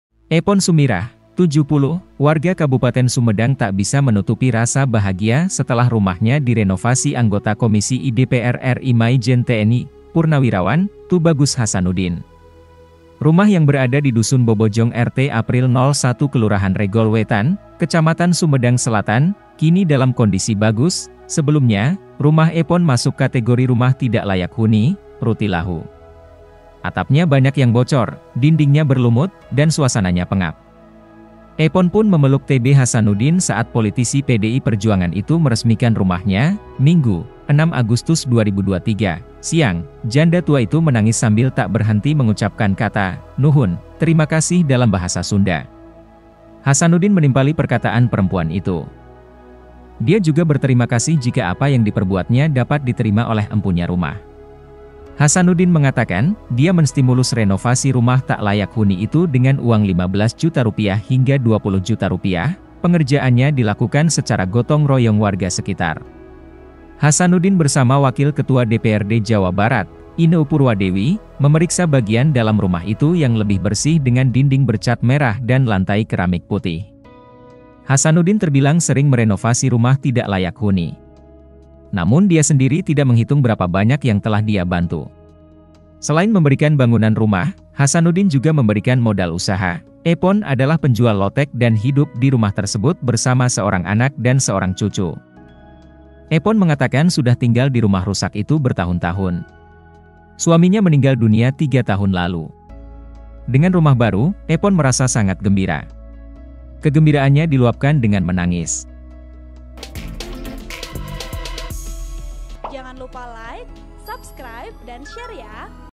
Bahagianya Penjual Lotek di Sumedang Setelah Rumahnya Direnovasi TB Hasanuddin, Tak Henti Ucap Nuhun
Artikel ini telah tayang di TribunJabar.id dengan judul Bahagianya Penjual Lotek di Sumedang Setelah Rumahnya Direnovasi TB Hasanuddin, Tak Henti Ucap Nuhun,
Penulis: Kiki Andriana | Editor: Giri |Video Editor: Uben